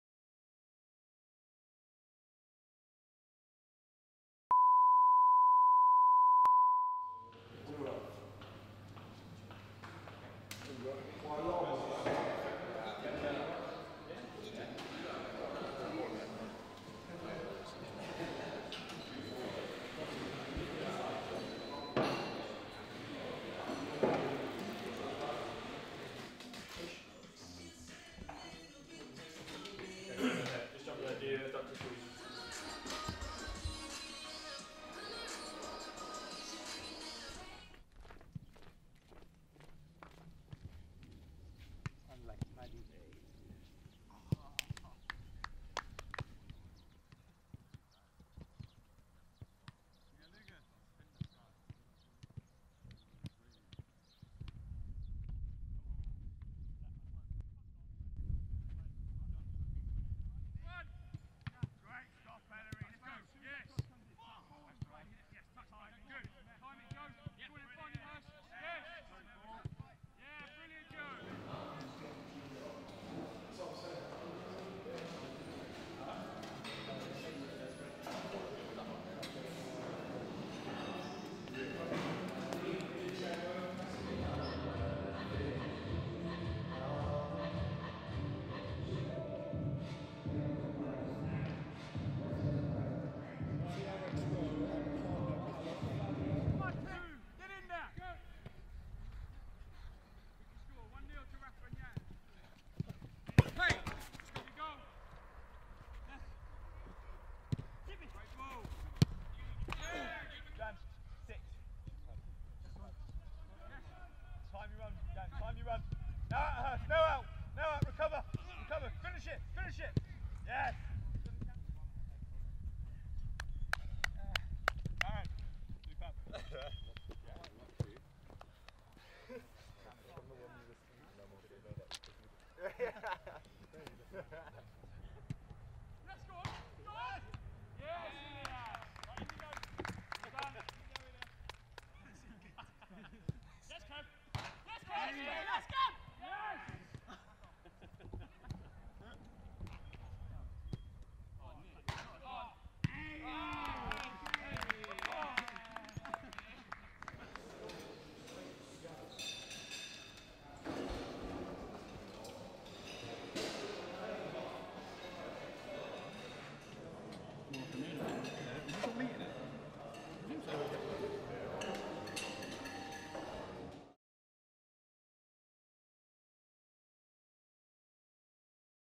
Hungary Uncovered
A look at a typical B Team day on pre season tour in Hungary.